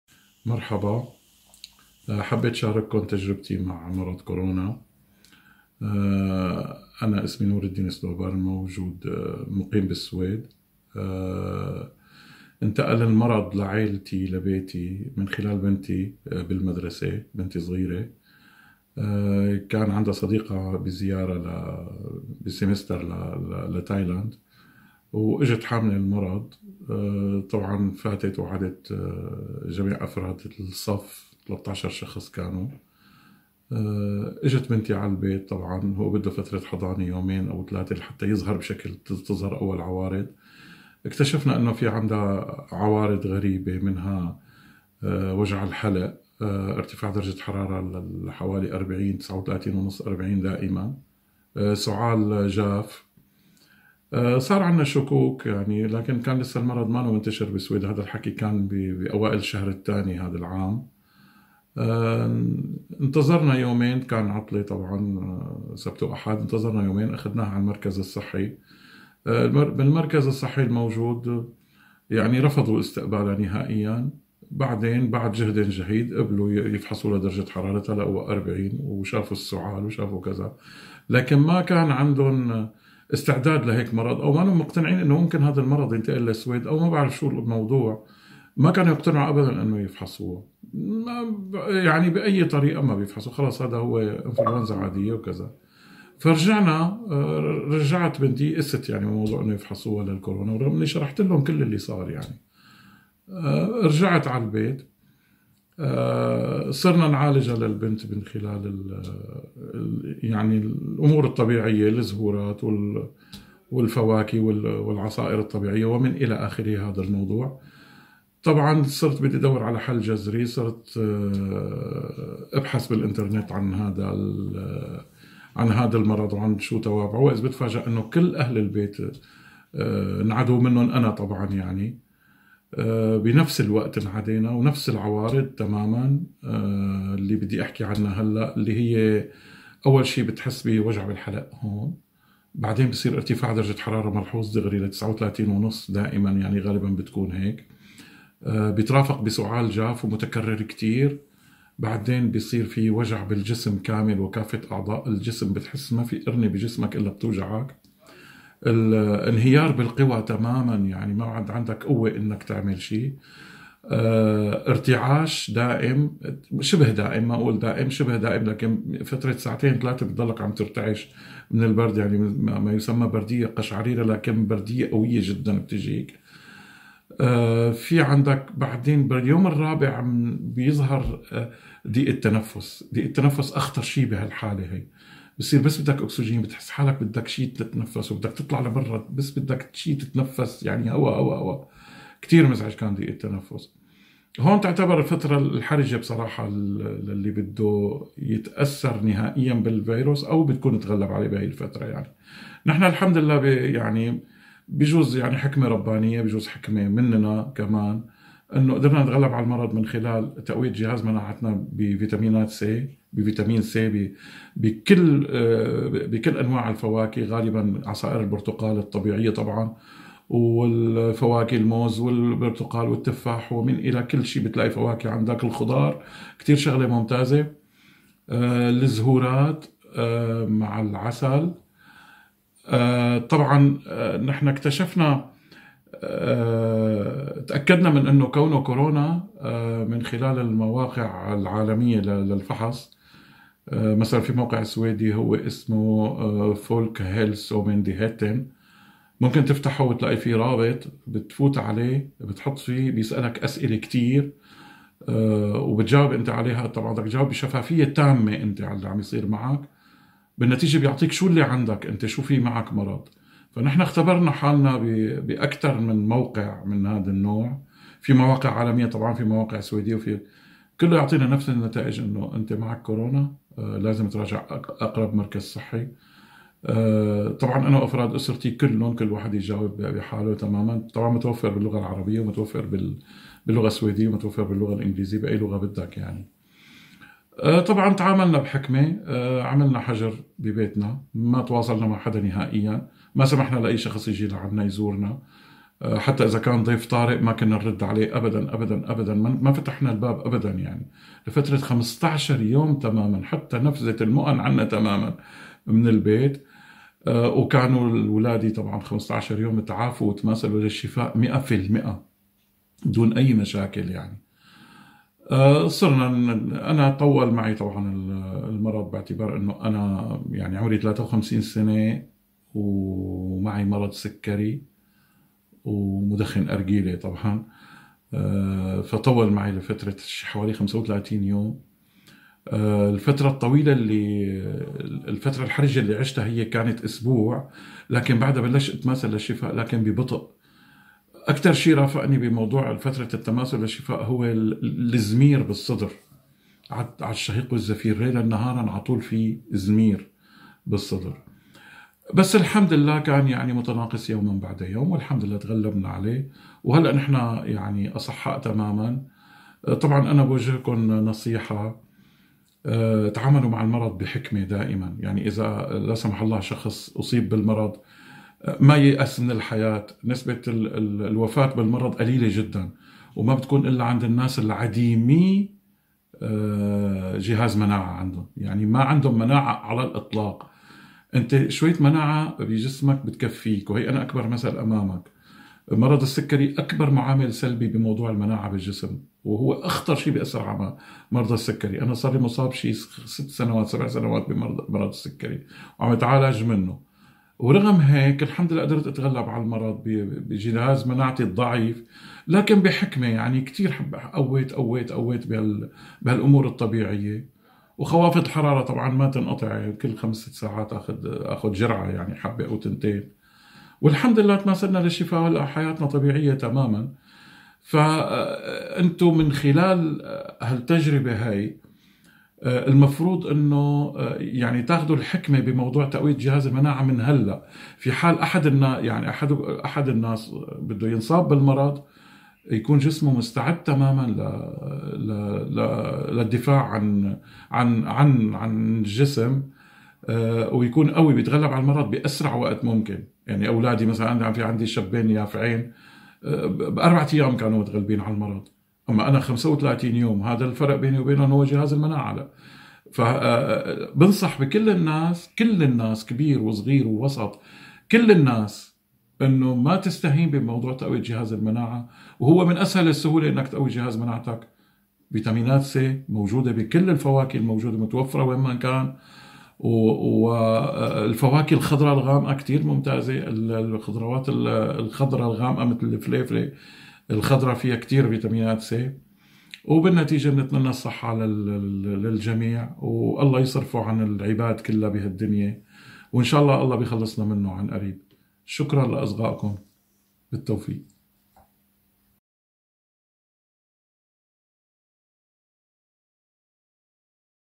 متعافٍ يروي لـ"الكومبس" قصته مع كورونا
شاهد( خاص من تلفزيون الكومبس): نور الدين سنوبر (53 عاماً) من سكان جنوب السويد. عاش وأفراد أسرته الخمسة تجربة مرض كورونا طيلة شهر كامل تقريباً.
تجربة يرويها نور الدين بنفسه..